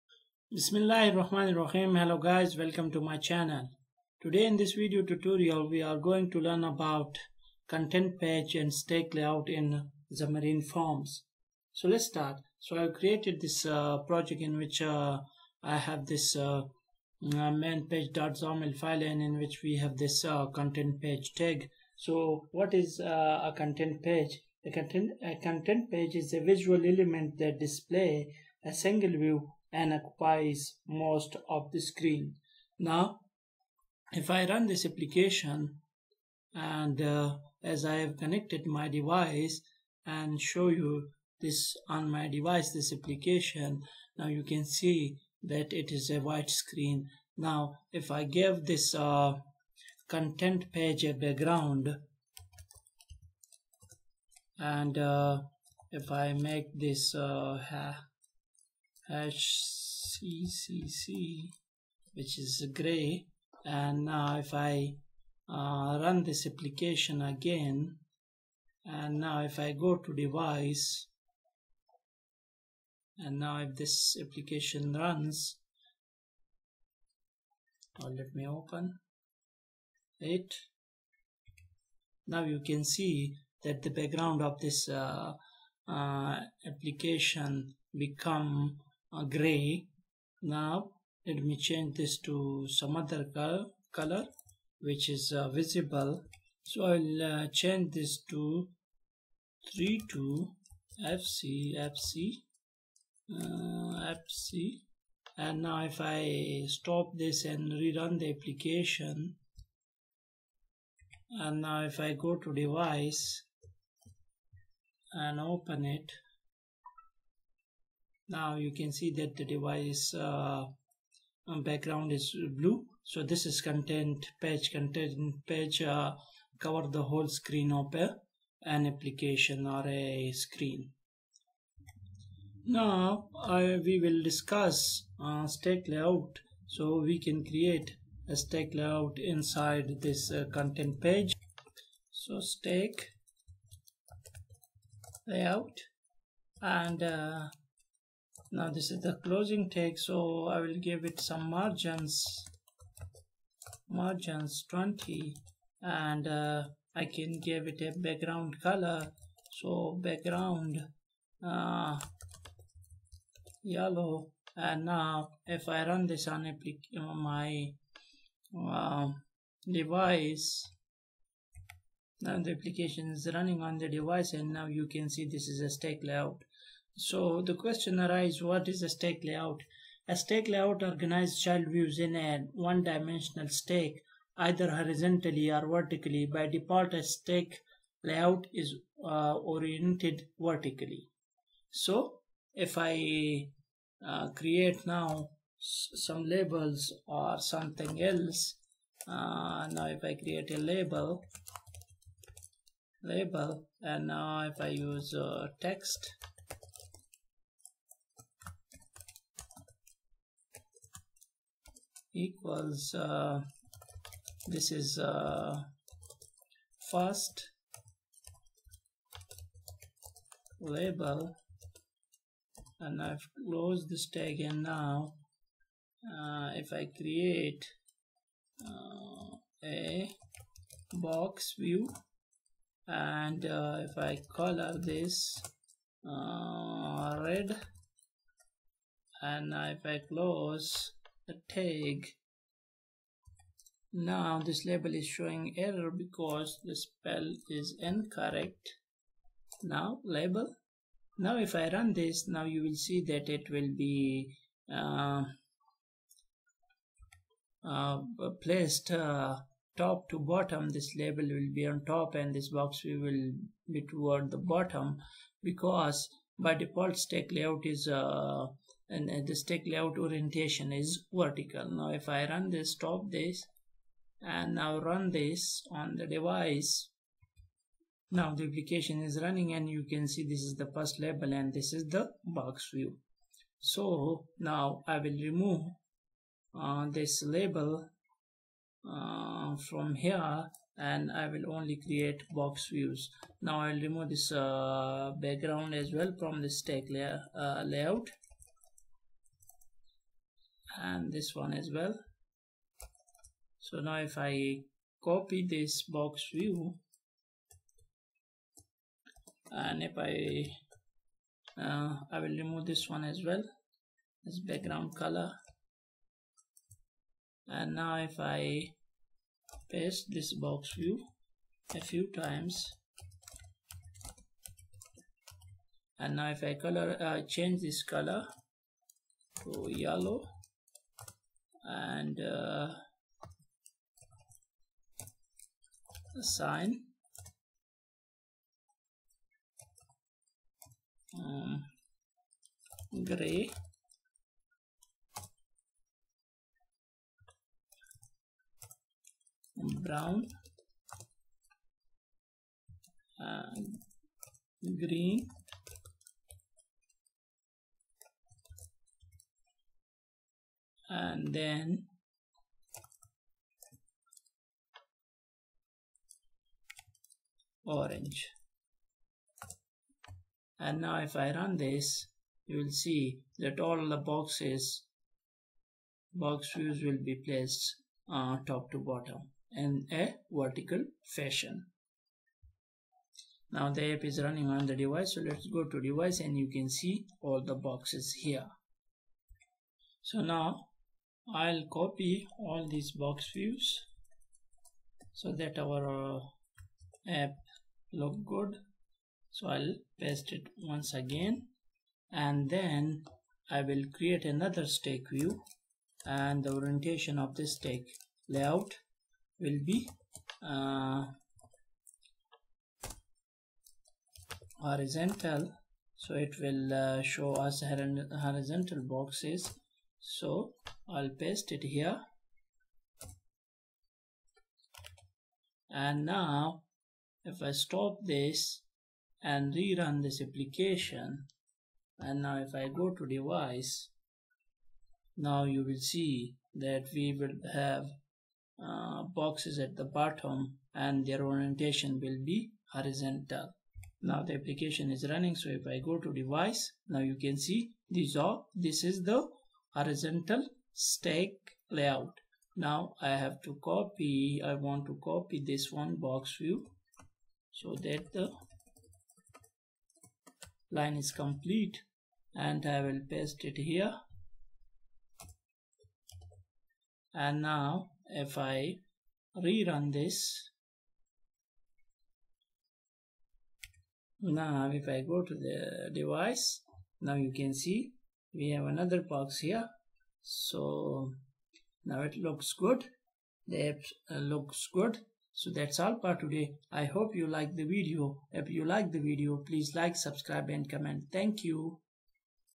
Stack Layout and Content Page in xamarin forms | Xamarin StackLayout and ContentPage
A StackLayout organizes child views in a one-dimensional stack, either horizontally or vertically. By default, a StackLayout is oriented vertically. In addition, a StackLayout can be used as a parent layout that contains other child layouts.

The StackLayout class defines the following properties:

    Orientation, of type StackOrientation, represents the direction in which child views are positioned. The default value of this property is Vertical.
    Spacing, of type double, indicates the amount of space between each child view. The default value of this property is six device-independent units.

ContentPage
ContentPage is the simplest and most common type of page. Set the Content property to a single View object, which is most often a Layout such as StackLayout, Grid, or ScrollView.

For C# Gui tutorial follow the Following link